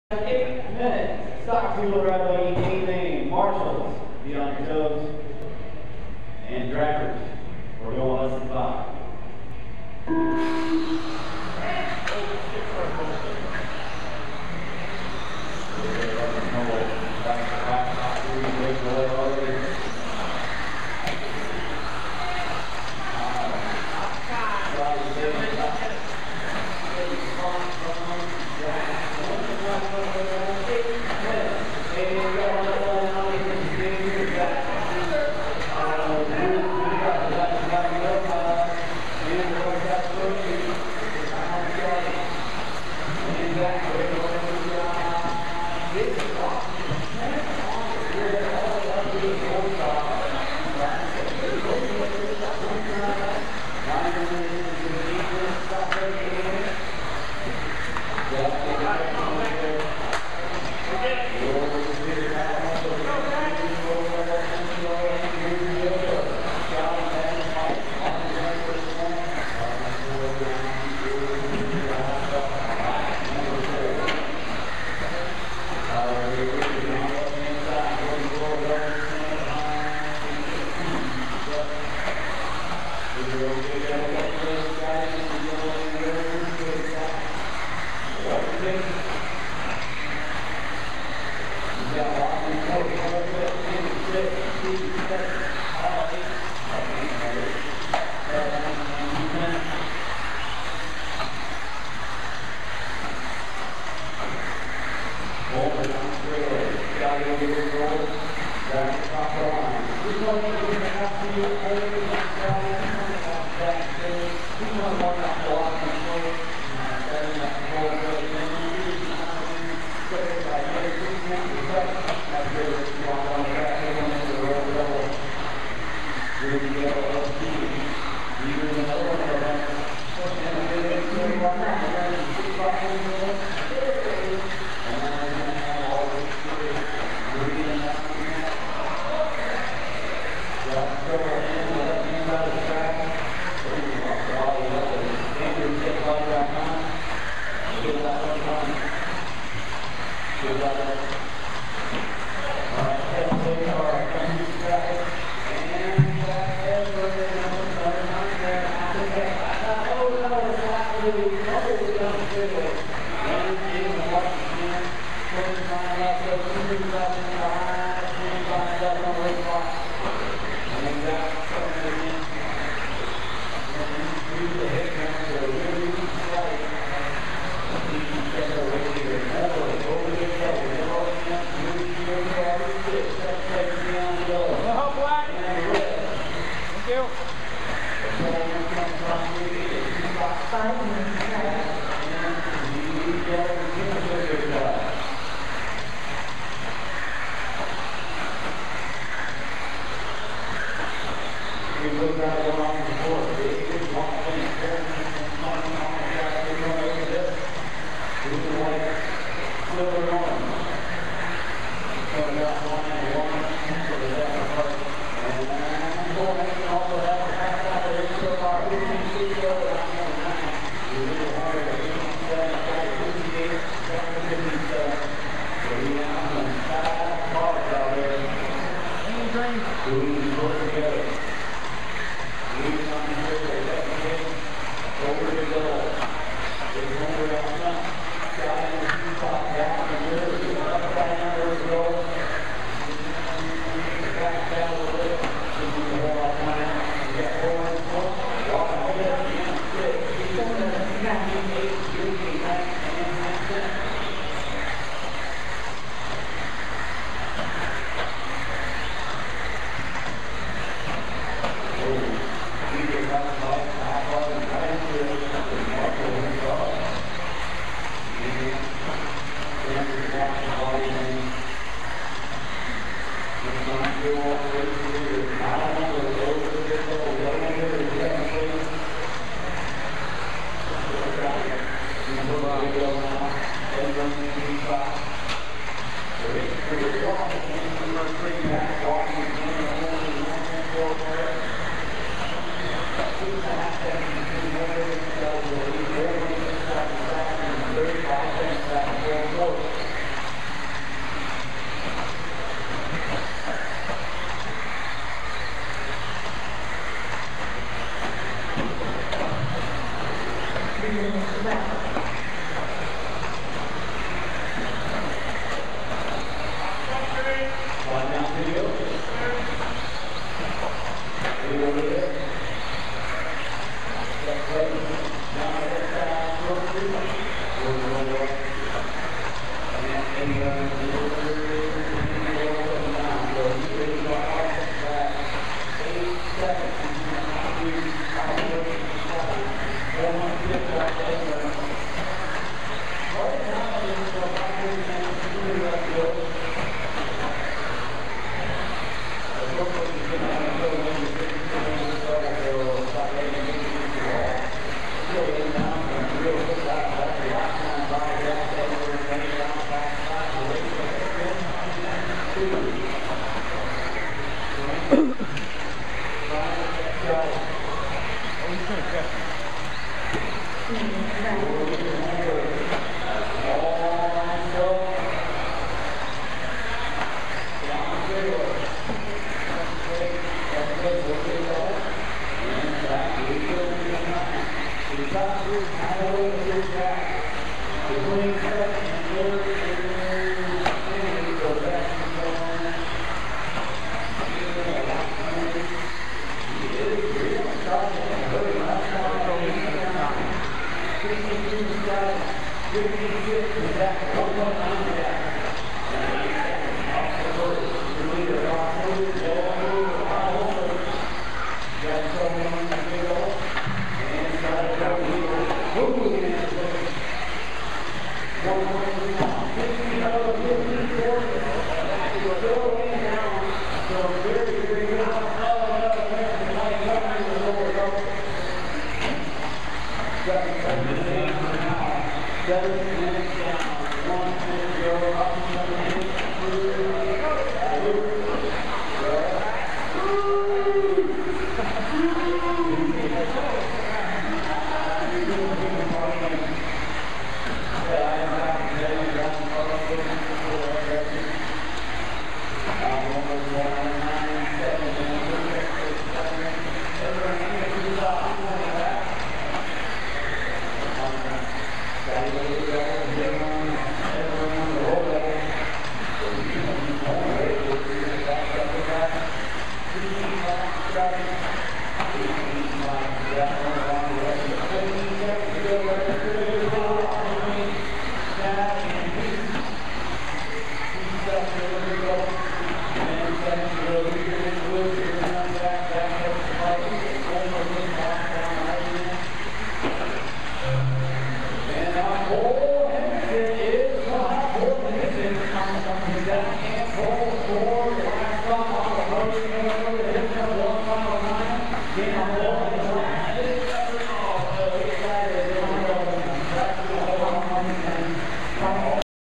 2015 BGRC Summer Smackdown Stock 2wd Buggy A Main
2015 BGRC Summer Smackdown

This event ran from 9/4 to 9/6 at Bluegroove Hobbies & Raceway